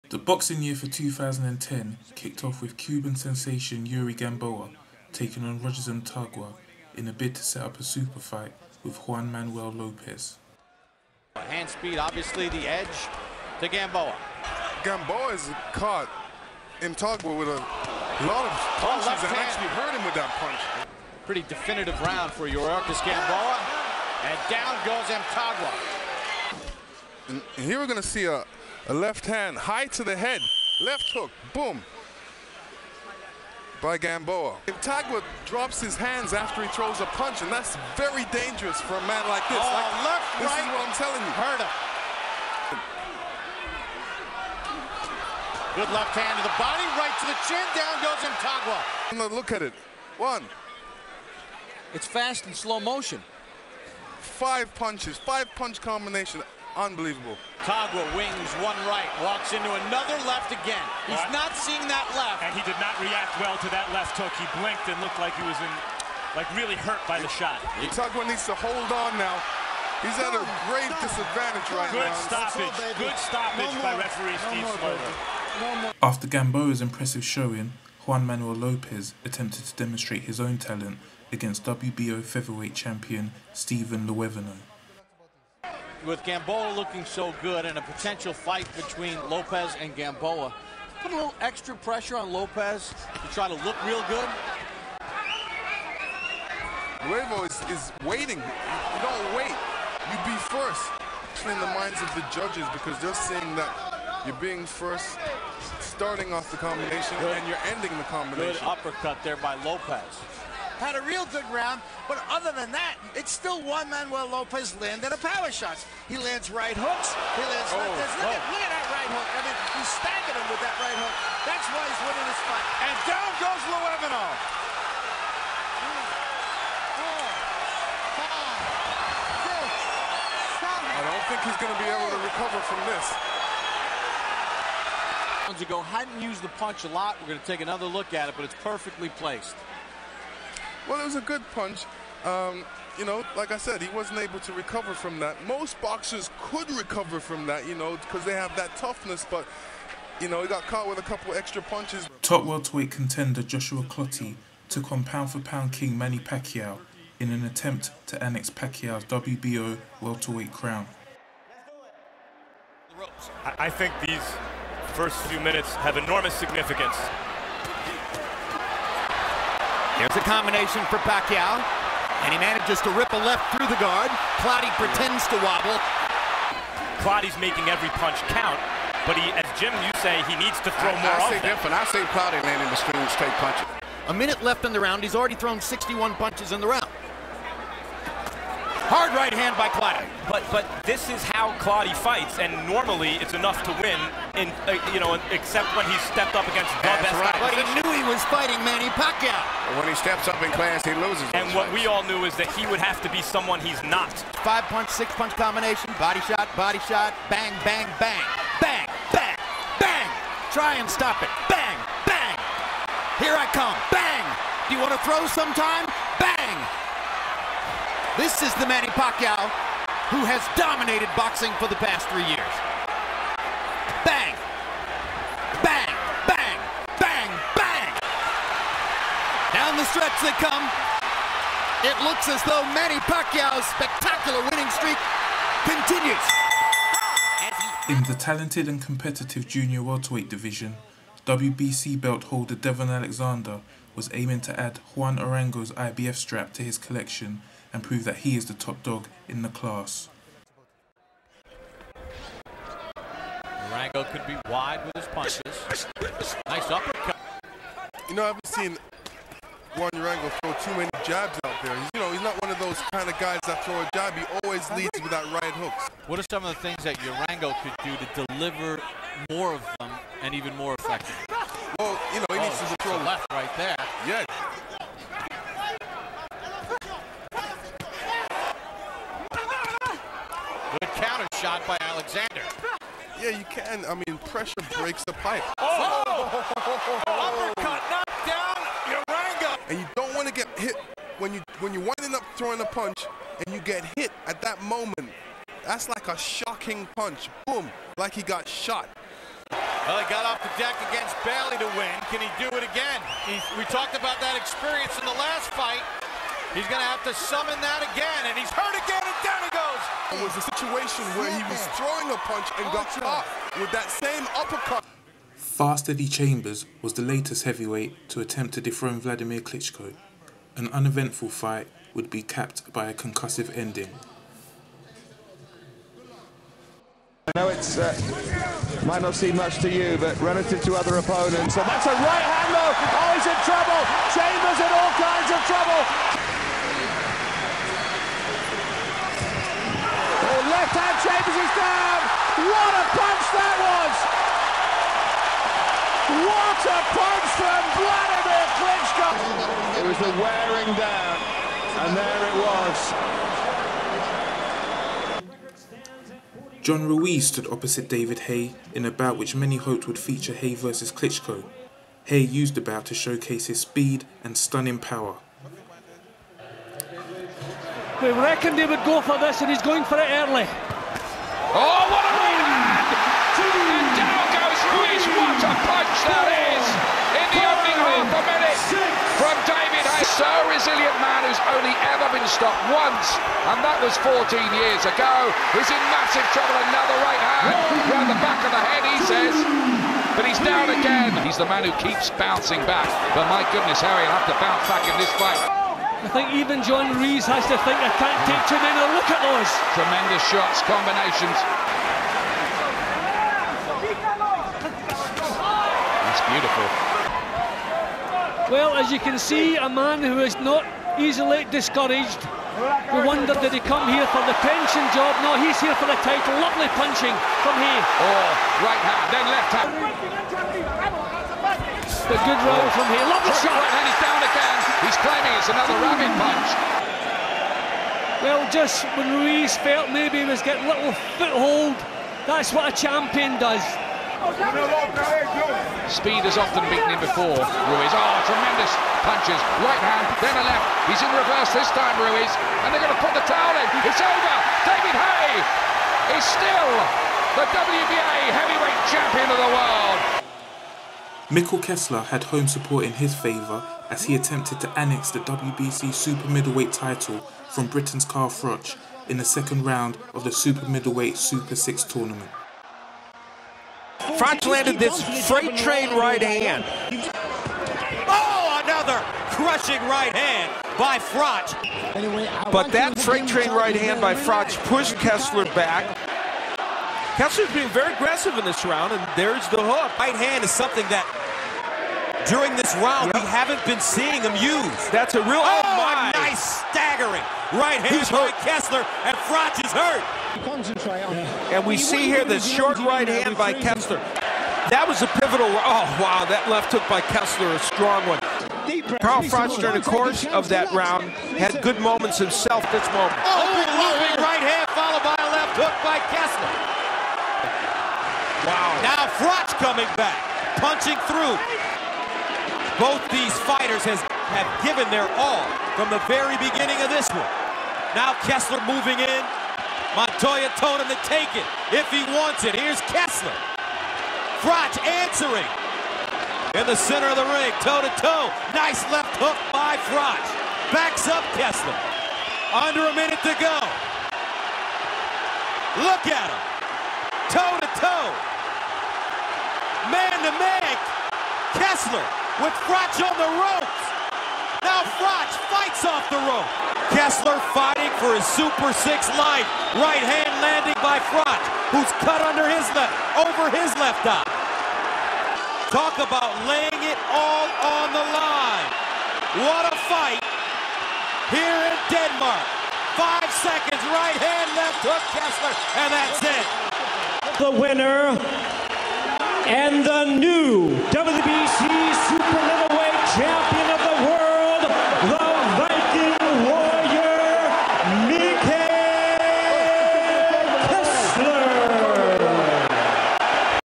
Boxing Review 2010 part 1
The first part of the Boxing Review for 2010 features a couple of showcases for the featherweights with Yuriorkis Gamboa v Rogers Mtagwa and Juan Manuel Lopez v Steven Luevano. Devon Alexander demonstrates his power v Juan Urango. Manny Pacquiao puts on a masterclass v Joshua Clottey. David Haye brutalizes John Ruiz and Carl Froch wages war with Mikel Kessler in the Super 6 tournament